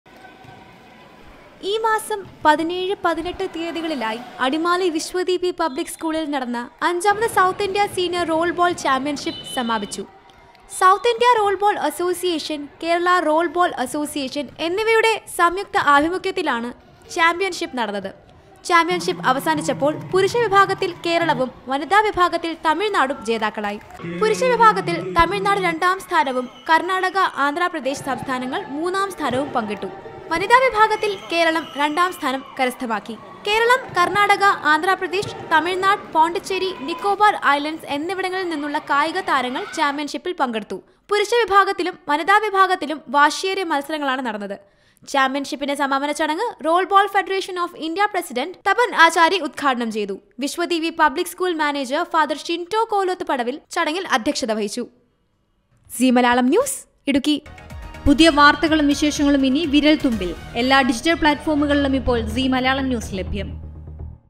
Roll Ball Championship 2024 | സീനിയര്‍ റോള്‍ബോള്‍ ചാമ്പ്യന്‍ഷിപ്പ് പുരുഷ വിഭാഗത്തില്‍ കേളം ജേതാക്കൾ
Roll Ball Championship 2024 | അടിമാലിയില്‍ നടന്നുവന്നിരുന്ന അഞ്ചാമത് സൗത്ത് ഇന്ത്യ സീനിയര്‍ റോള്‍ബോള്‍ ചാമ്പ്യന്‍ഷിപ്പ് പുരുഷ വിഭാഗത്തില്‍ കേരളം ജേതാക്കൾ